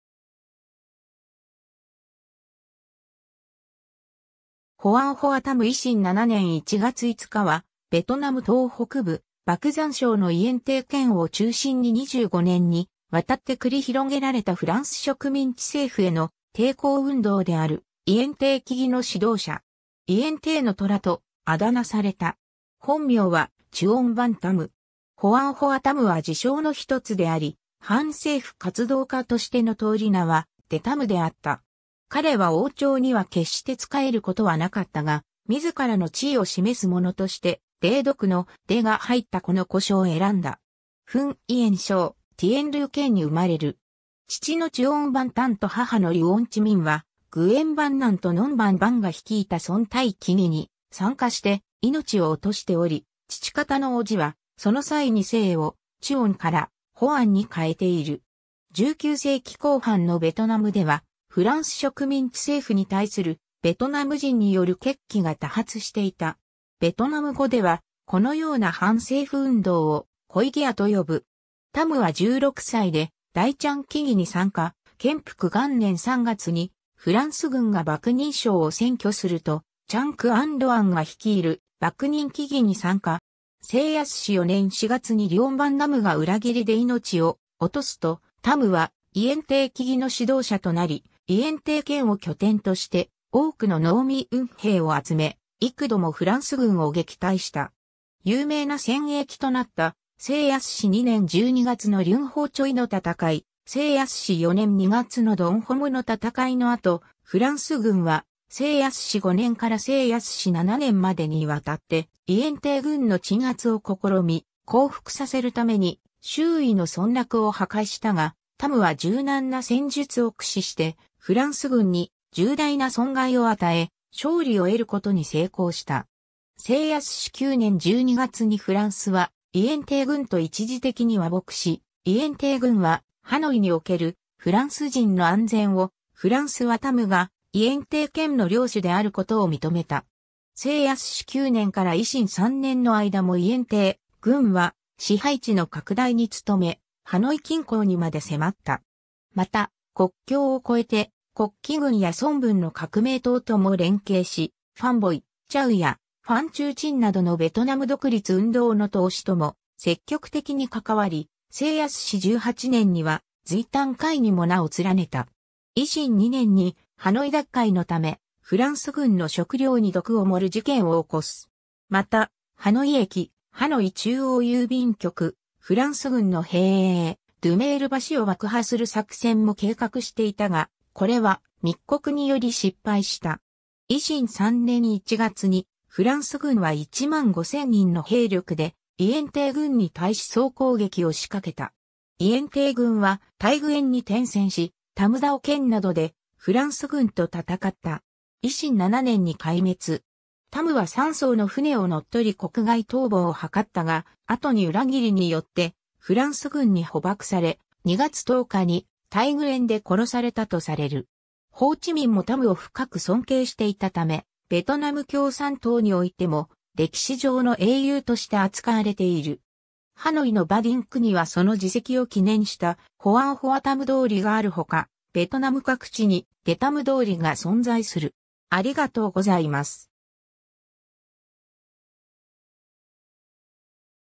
ホアン・ホア・タム
ホアン・ホア・タム（ベトナム語：Hoàng Hoa Thám/ 黃花探、嗣徳11年（1858年） - 維新7年1月5日（1913年2月10日））は、ベトナム東北部・バクザン省のイエンテー県を中心に25年にわたって繰り広げられたフランス植民地政府への抵抗運動であるイエンテー起義の指導者。
「イエンテーの虎」（ベトナム語: Hùm thiêng Yên Thế）と渾名された。
 本名はチュオン・ヴァン・タム（ベトナム語：Trương Văn Thám/ 張文探）。
ホアン・ホア・タムは自称の一つであり、反政府活動家としての通り名はデ・タム（ベトナム語：Đề Thám/ 提探）であった。
彼は王朝には決して仕えることはなかったが、自らの地位を示すものとして「デードク」（ベトナム語：Đề đốc/ 提督）の「デ」が入ったこの呼称を選んだ。
 フンイエン省ティエンルー県に生まれる。
父のチュオン・ヴァン・タン（ベトナム語：Trương Văn Thận/ 張文慎）と母のリュオン・チ・ミン（ベトナム語：Lương Thị Minh/ 梁氏明）は、グエン・ヴァン・ナン（ベトナム語：Nguyễn Văn Nhân/ 阮文雁）とノン・ヴァン・ヴァンが率いたソンタイ起義に参加して命を落としており、父方の叔父はその際に姓を「チュオン」（ベトナム語：Trương/ 張）から「ホアン」（ベトナム語：Hoàng/ 黃）に変えている。
 19世紀後半のベトナムでは、フランス植民地政府に対するベトナム人による決起が多発していた。
ベトナム語ではこのような反政府運動を「コイギア」（ベトナム語：khởi nghĩa/ 起義）と呼ぶ。
 タムは16歳でダイチャン起義（1870年 - 1875年）に参加、建福元年（1884年）3月にフランス軍がバクニン省を占拠すると、チャン・クァン・ロアン（ベトナム語：Trần Quang Loan/ 陳光巒）が率いるバクニン起義に参加、咸宜元年（1885年）にはホアン・ディン・キンの率いるランザン起義（1882年 - 1888年）に参加、ホアン・ディン・キンの死後は兵を率いて「デ・ナム」（ベトナム語：Đề Nắm/ 提楠）ことリュオン・ヴァン・ナム（ベトナム語：Lương Văn Nắm/ 梁文楠）の率いる起義軍の麾下に入り、指導者の一人として名を馳せた。
 成泰4年（1892年）4月にリュオン・ヴァン・ナムが裏切りで命を落とすと、タムはイエンテー起義の指導者となり、イエンテー県を拠点として多くの農民兵を集め、幾度もフランス軍を撃退した。
有名な戦役となった成泰2年（1890年）12月のリュンホーチョイの戦い、成泰4年（1892年）2月のドンホムの戦いの後、フランス軍は成泰5年（1893年）から成泰7年（1895年）までにわたってイエンテー軍の鎮圧を試み、降伏させるために周囲の村落を破壊したが、タムは柔軟な戦術を駆使してフランス軍に重大な損害を与え、勝利を得ることに成功した。
成泰9年（1897年）12月にフランスはイエンテー軍と一時的に和睦し、イエンテー軍はハノイにおけるフランス人の安全を、フランスはタムがイエンテー県の領主であることを認めた。
 成泰9年（1897年）から維新3年（1909年）の間もイエンテー軍は支配地の拡大に努め、ハノイ近郊にまで迫った。
また、国境を越えて黒旗軍や孫文の革命党とも連携し、ファン・ボイ・チャウやファン・チュー・チンなどのベトナム独立運動の闘士とも積極的に関わり、成泰18年（1906年）にはズイタン会にも名を連ねた。
 維新2年（1908年）にハノイ奪回のため、フランス軍の食料に毒を盛る事件を起こす（ハノイ投毒事件）。
またハノイ駅、ハノイ中央郵便局、フランス軍の兵営、ドゥメール橋（現在のロンビエン橋）を爆破する作戦も計画していたが、これは密告により失敗した。
維新3年（1909年）1月にフランス軍は15,000人の兵力でイエンテー軍に対し総攻撃を仕掛けた。
イエンテー軍はタイグエンに転戦し、タムダオ県などでフランス軍と戦った。
維新7年（1913年）に壊滅。
タムは三艘の船を乗っ取り国外逃亡を図ったが、後に裏切りによってフランス軍に捕縛され、2月10日にタイグエンで殺されたとされる。
 ホー・チ・ミンもタムを深く尊敬していたため、ベトナム共産党においても歴史上の英雄として扱われている。
ハノイのバディン区にはその事跡を記念した「ホアン・ホア・タム通り」...